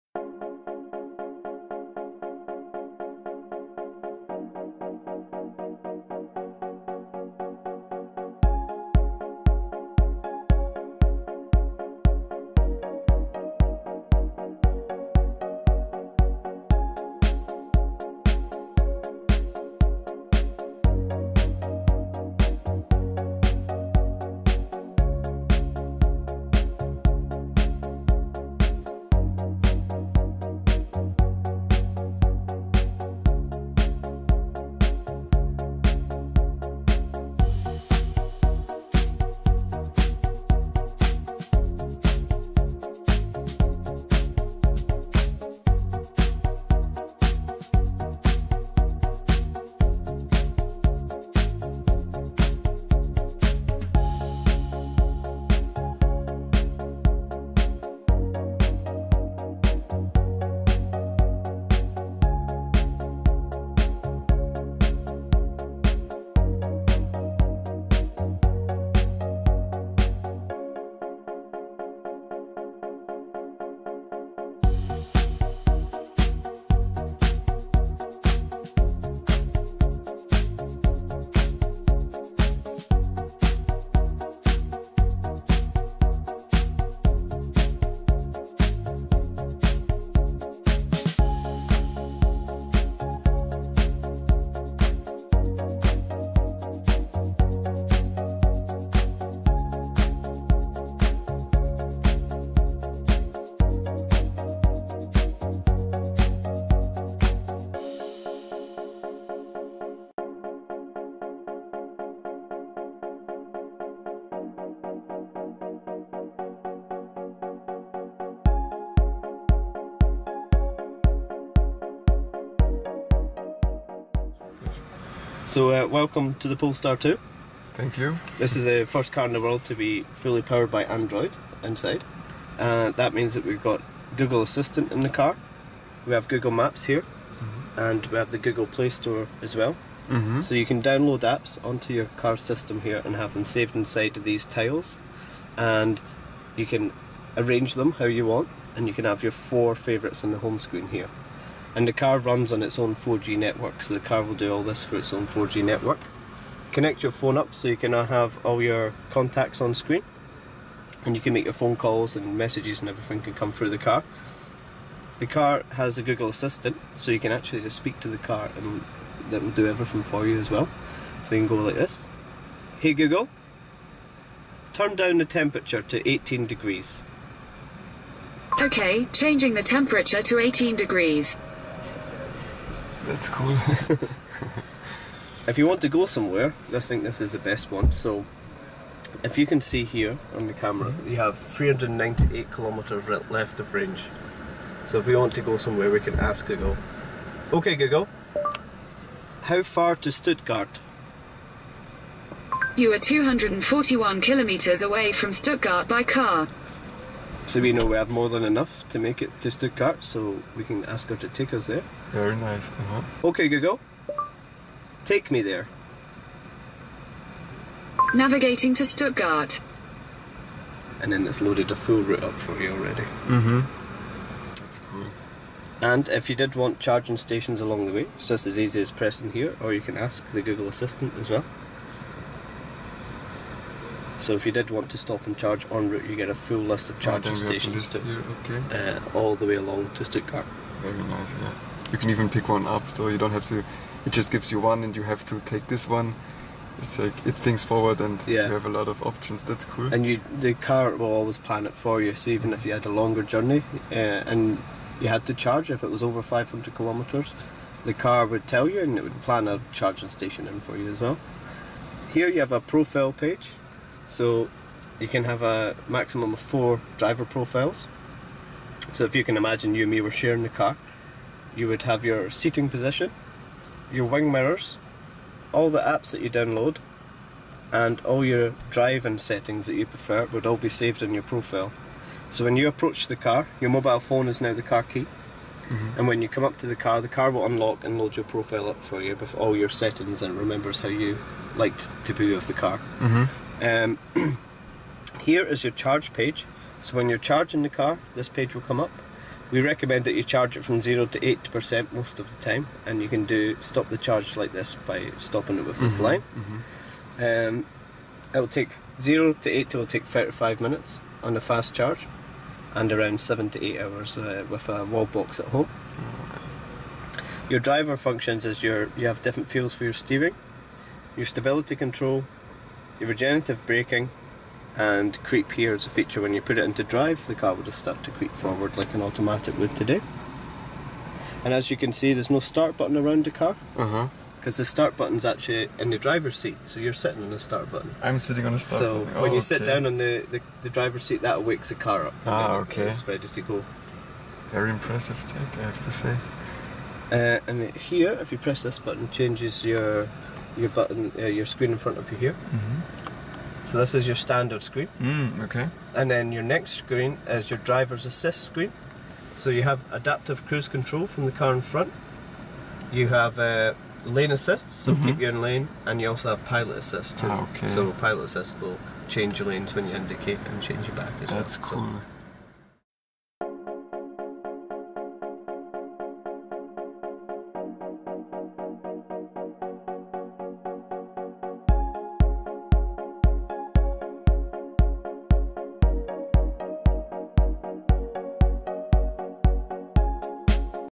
Volvo Polestar 2: Walk-Around Interior, Exterior, Infotainment
Walkaround Point of View (POV) Interior and Exterior Video of the new Volvo Polestar 2.
Starting in Mid 2020 with 500 km (WLTP) Range.

Car was displayed at the Munich International Airport (MUC), Germany